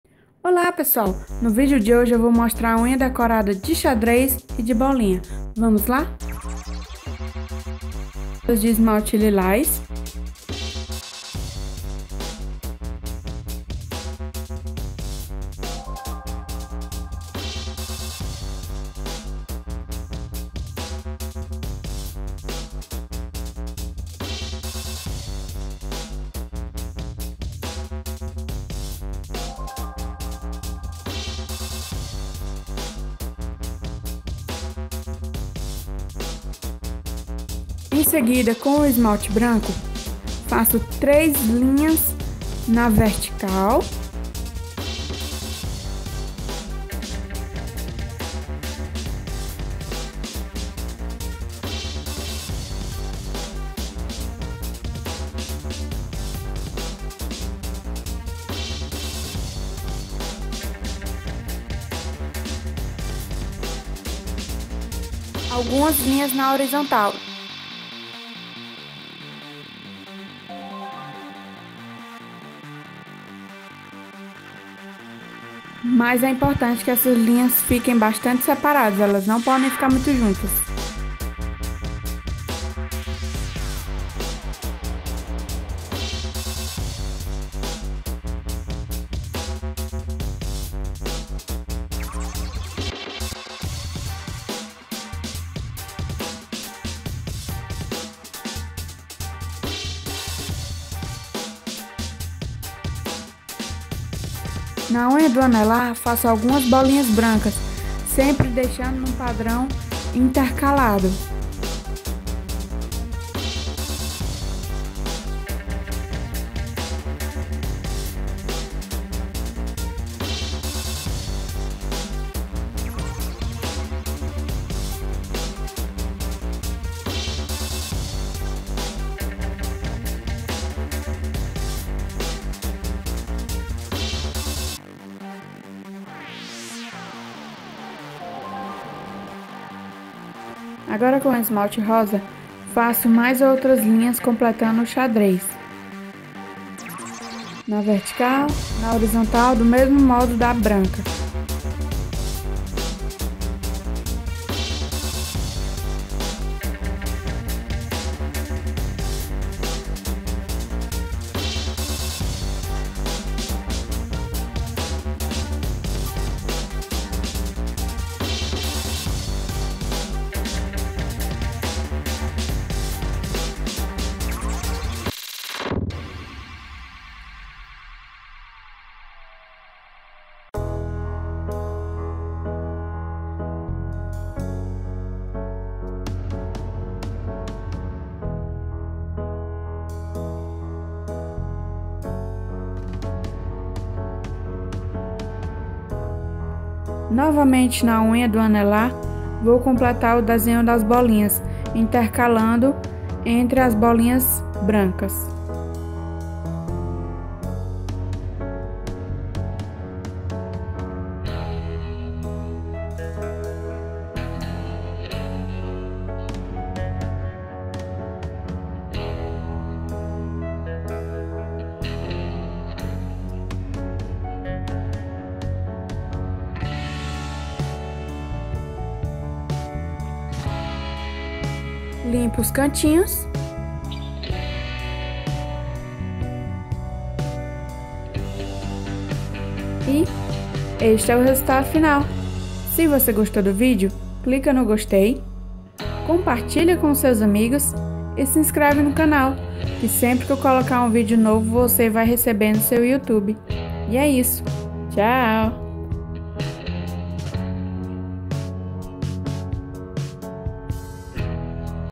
Unha decorada xadrez e bolinha
Olá Pessoal, no vídeo de hoje trago uma decoração de unha de xadez e bolinha, vamos lá?